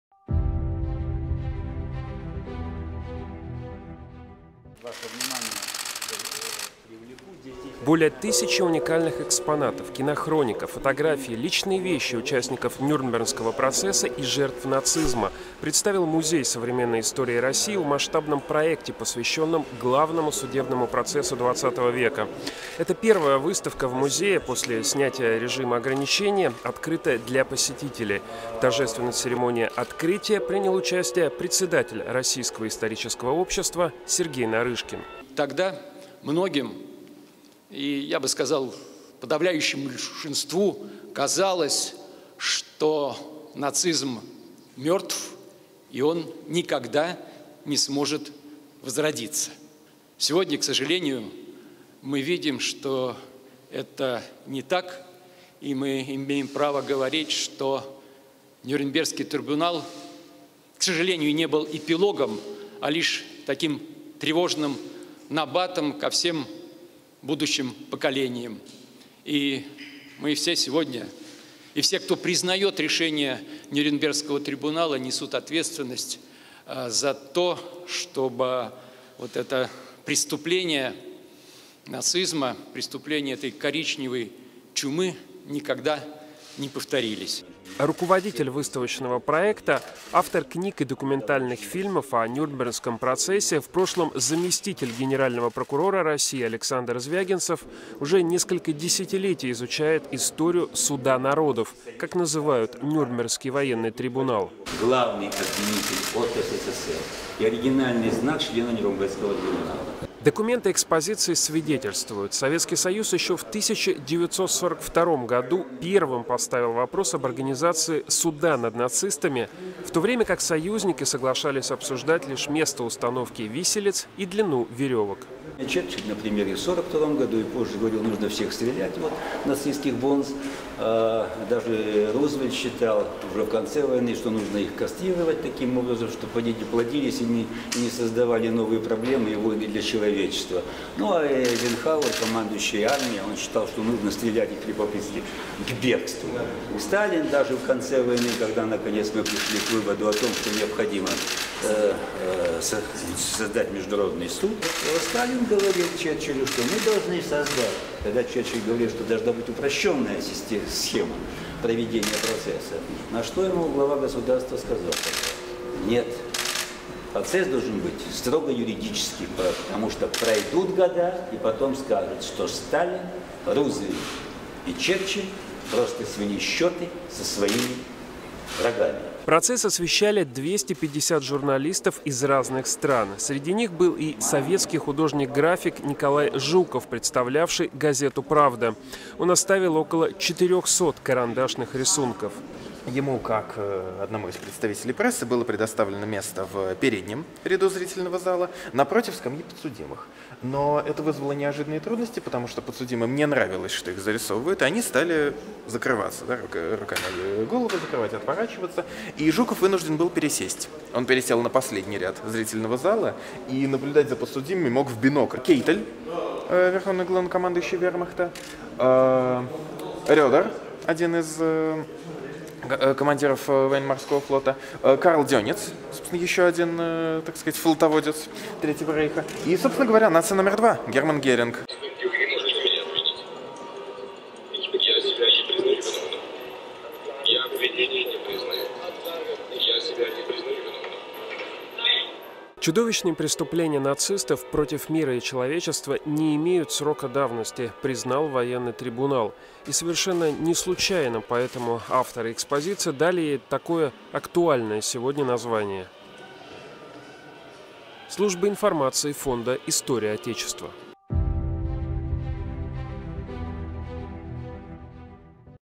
В ГЦМСИР открылась выставка к 75 летию начала «Суда народов»
В Государственном центральном музее современной истории России открылся для посетителей масштабный историко-документальный просветительский выставочный проект, посвящённый главному судебному процессу XX века – «Нюрнбергский набат. Без срока давности».
Проект реализован при поддержке Фонда президентских грантов.
Судебный процесс над высшими государственными и военными деятелями нацистской Германии проходил в г. Нюрнберге (Германия) с 20 ноября 1945 по 1 октября 1946 г. в Международном военном трибунале. Трибунал был сформирован на паритетных началах из представителей четырех великих держав (СССР, США, Великобритания, Франция) на конференции 23 государств, подписавших 8 августа 1945 г. в Лондоне соглашение о судебном преследовании и наказании главных военных преступников. Процесс был гласным (все 403 судебных заседания были открытыми). С твёрдой позицией об открытости и публичности международного суда над нацистскими преступниками выступило советское руководство. Членом Трибунала от СССР являлся заместитель председателя Верховного Суда СССР генерал-майор юстиции Иона Никитченко. Главным обвинителем от СССР выступал Роман Руденко (прокурор Украинской ССР, впоследствии Генеральный прокурор СССР). Вещи из личного собрания его внука вошли в состав открывшейся экспозиции. Всего в рамках проекта представлено более тысячи экспонатов. Среди них архивные документы, фото- и кинохроника, а также личные вещи жертв фашистских палачей.